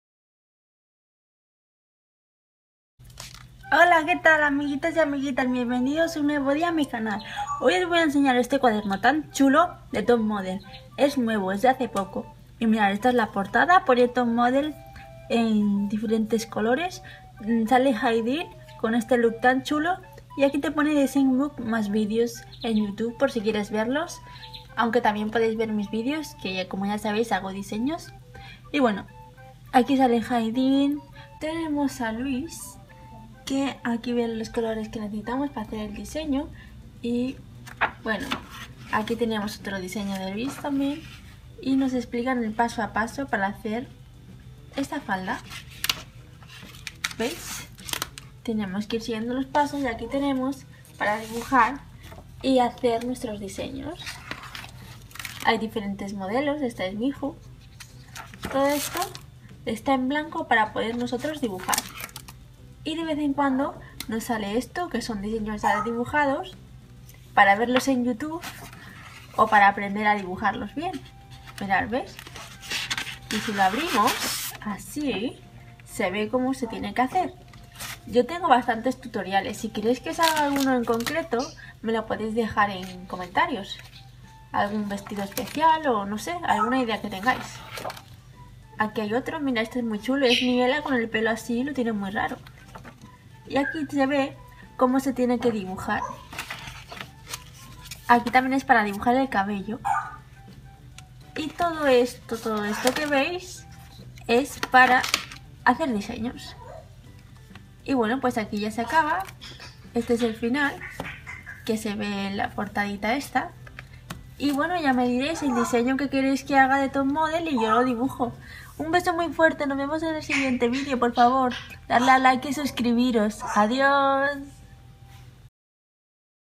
Cuaderno TopModel 2016
Nuevo cuaderno para hacer dibujos y diseños de moda a las modelos de TopModelBiz.